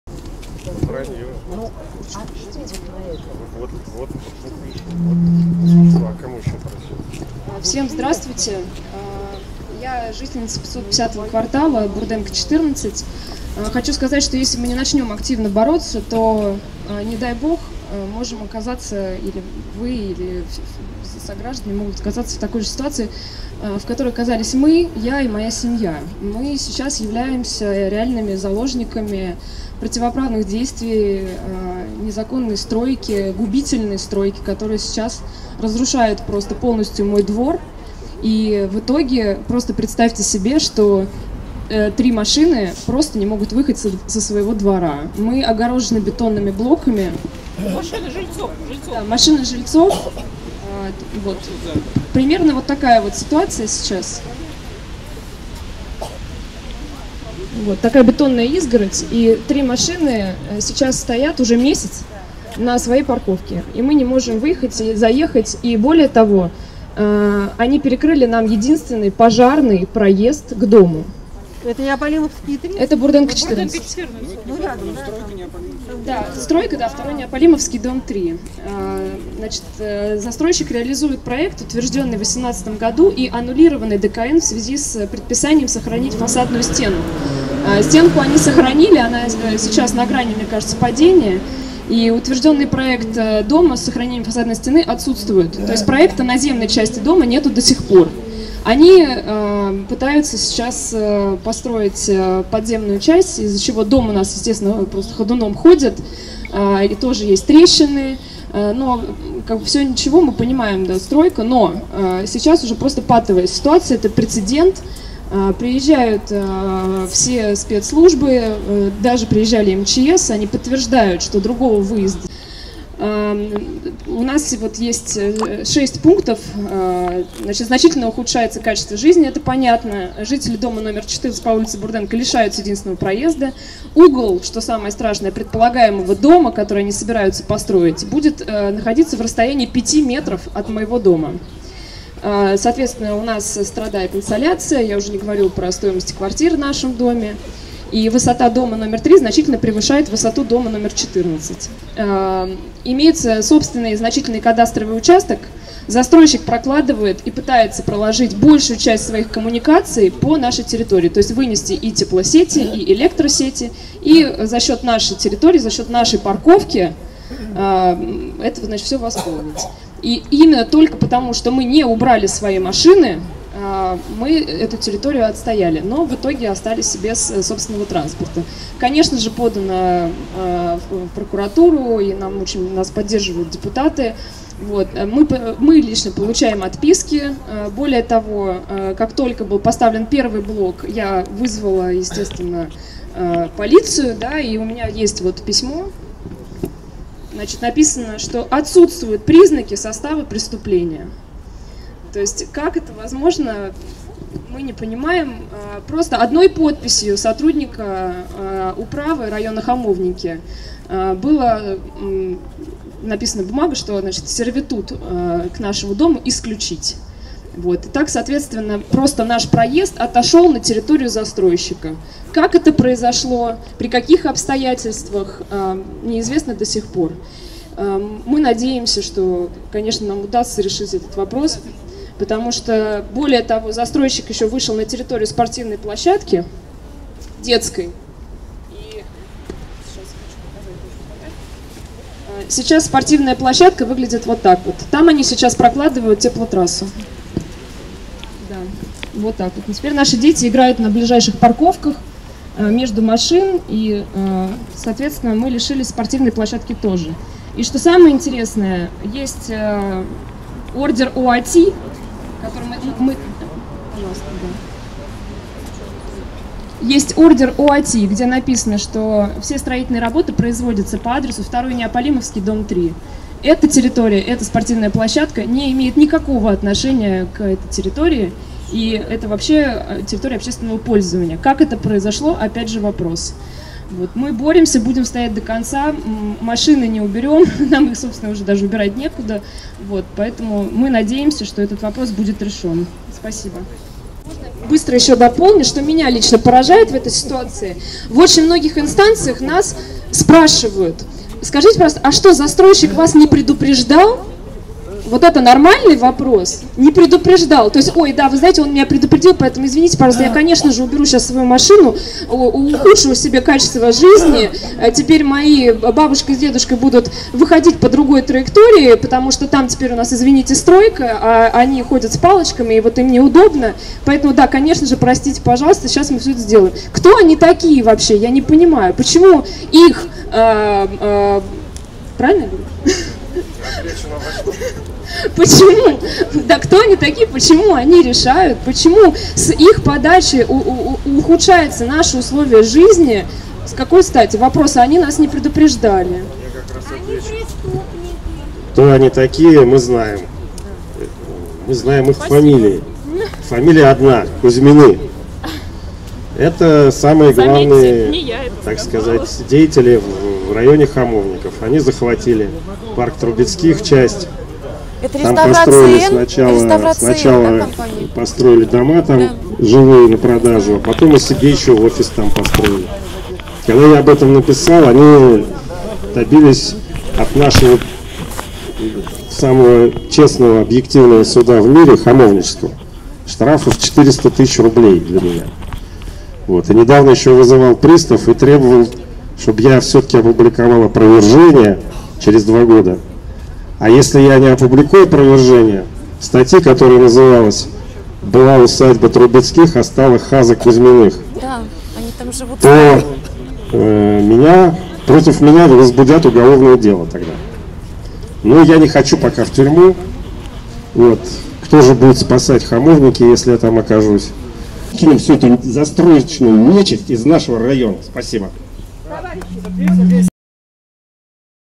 АНАСТАСИЯ ПРОТИВ НЕДОБРОСОВЕСТНЫХ ЗАСТРОЙЩИКОВ ПО БУРДЕНКО 14: «КТО ОНИ ТАКИЕ?!»
«Кто они такие? Почему решают за нас и портят качество нашей жизни???» Возмущалась Анастасия в адрес коммерческого застройщика, пришедшего на территорию их дома.На встрече жителей Хамовников с Муниципальными депутатами выступила Анастасия, жительница по ул.Бурденко 14, пострадавшая от застройщиков, из-за которых их жилой дом был обнесён строительным забором, и несколько машин жителей оказались заблокированными, также перекрыт доступ въезду аварийных служб.
АВТОР ВИДЕОСЪЁМКИ ИМОНТАЖА, ТАКЖЕ НЕ РАВНОДУШНЫЙ ЖИТЕЛЬ-Я, ОЛЬГА Н...